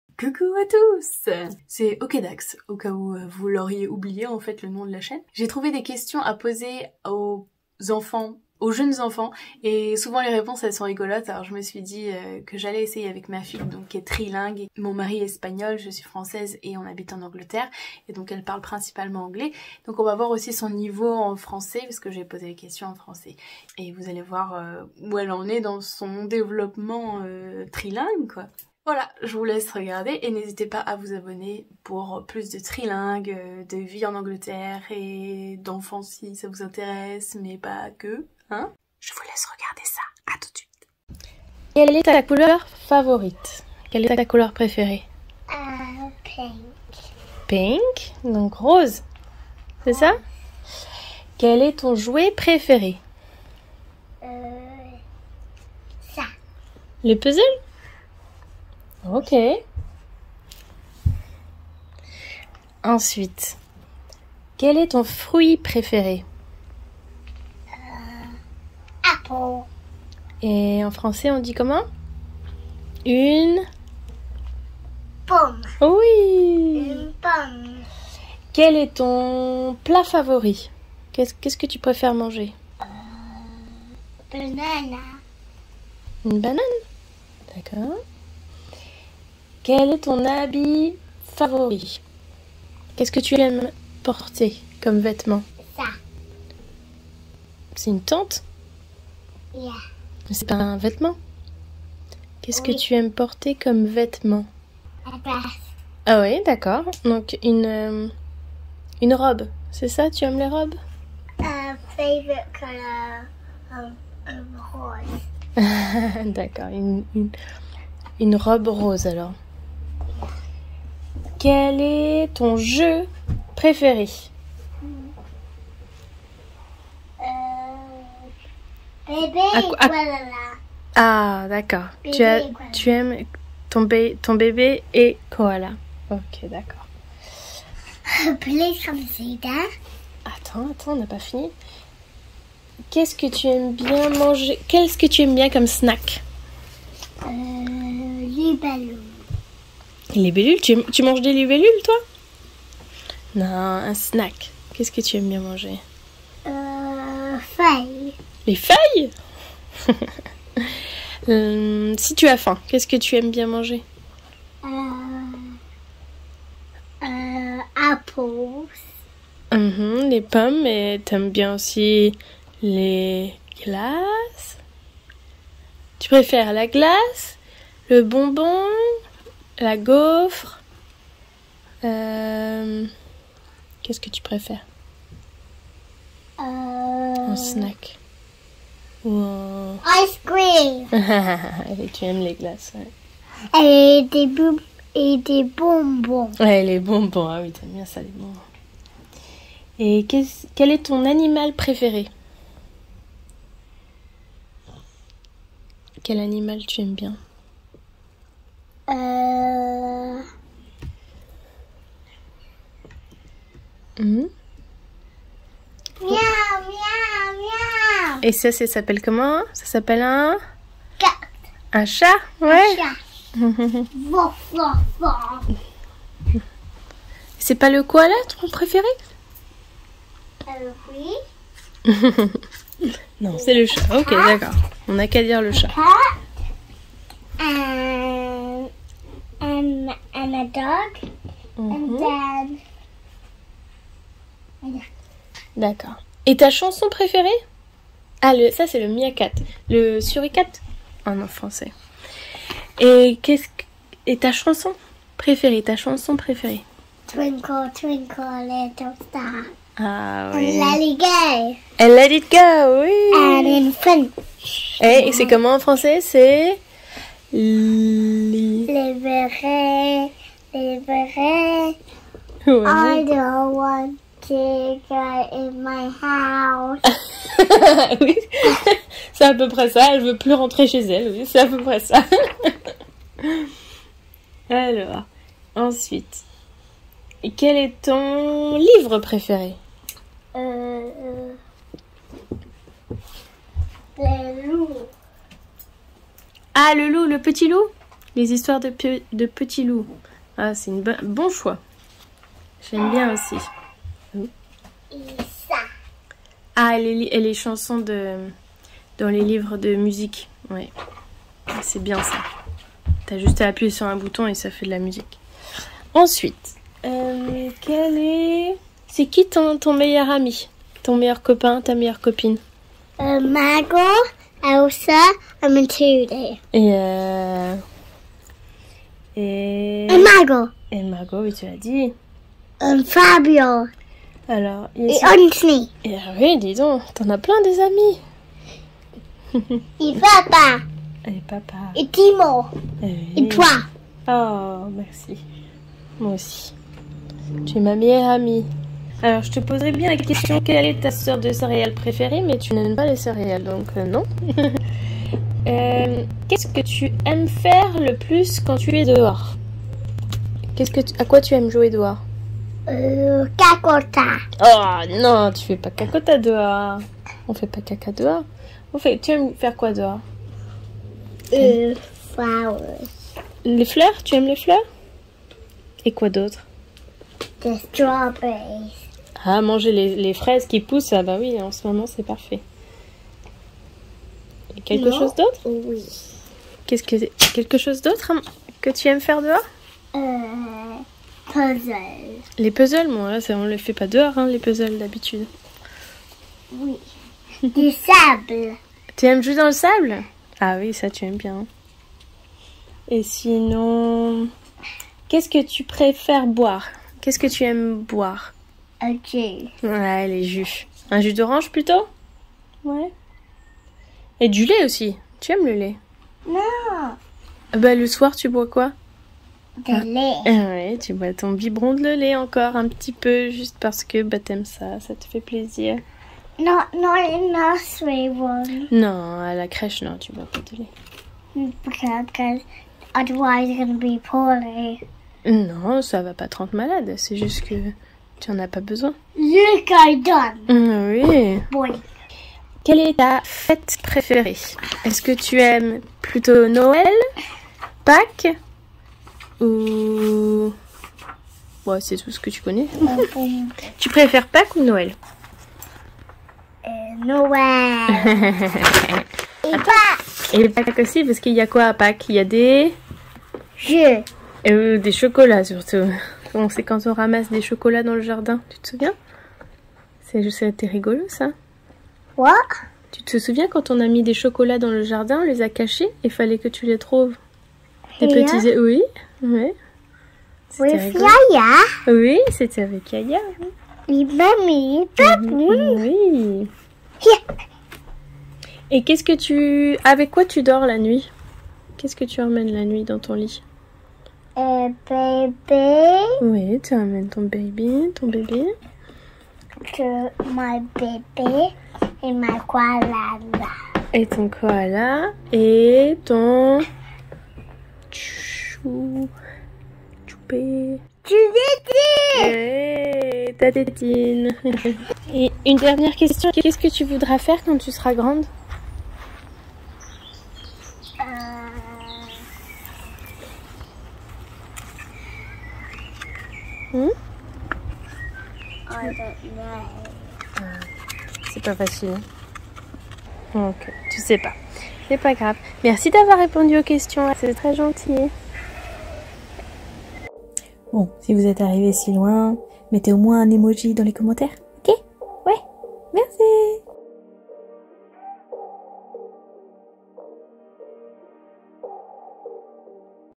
Comment parle un enfant TRILINGUE de 3 ans ? ENFANT FRANCO ANGLAIS
enfant hispano francais / enfant franco anglais

1. Quelle est ta couleur favorite?
2. Quel est ton jouet préféré?
3. Quel est ton fruit préféré?
4. Quel est ton dessin animé préféré ?
5. Quel est ton met favori?
6. Quel est ton habit-habillement favori?
7. Quel est ton jeu préféré?
8. Quelle est ta collation préférée?
9. Quel est ton animal favori?
10. Quelle est ta chanson préférée?
11. Quel est ton livre/histoire favorite?
12. Qui est ton meilleur ami?
13. Quelle est ta sorte de céréale préférée?
14. Qu’est ce que tu aimes le plus faire dehors?
15. Qu’est-ce que tu préfères boire?
16. Quelle est ta fête préférée?
17. Qu’est ce que tu aimes amener avec toi lorsque tu vas faire dodo?
18. Que veux-tu devenir/faire lorsque tu seras grand?